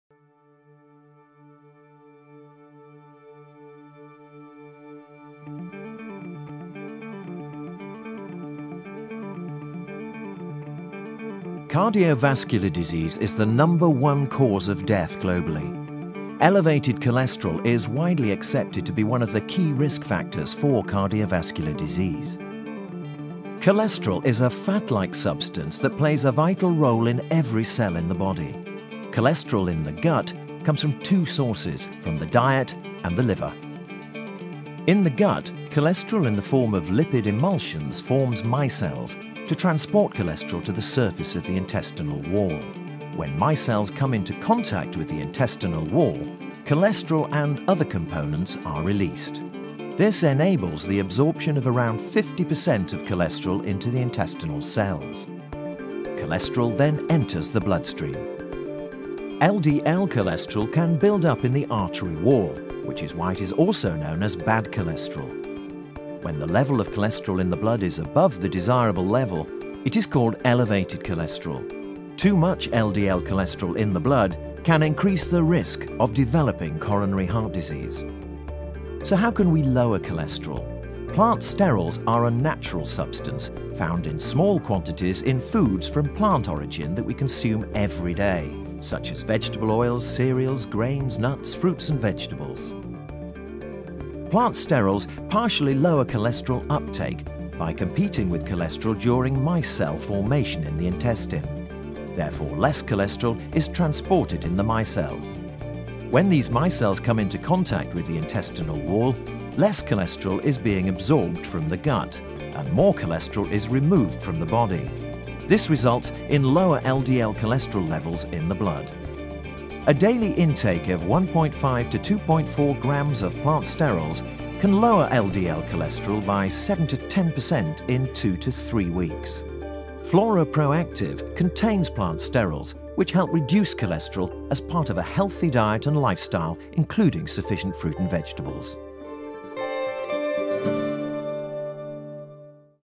Lowering Cholesterol - MoA Animation
Cardiovascular disease is the number one cause of death globally. Elevated cholesterol is widely accepted to be one of the key risk factors for cardiovascular disease. Cholesterol is a fat like substance that plays a vital role in every cell in the body. Cholesterol in the gut comes from two sources, from the diet and the liver.

So how can we lower cholesterol? Plant sterols are a natural substance found as part of the daily diet, in food of plant origin such as vegetable oils, cereals, grain, nut, fruit and vegetables.